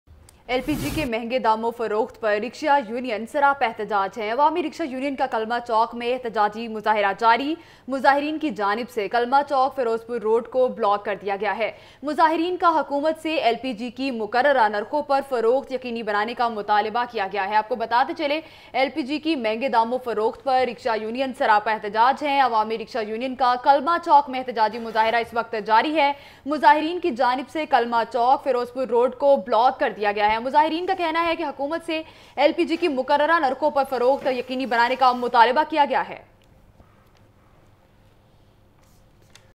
Rickshaw Union stages protest at Kalma Chowk due to increase in LPG prices
Rickshaw Union stage protest at Kalma Chowk due to increase in LPG prices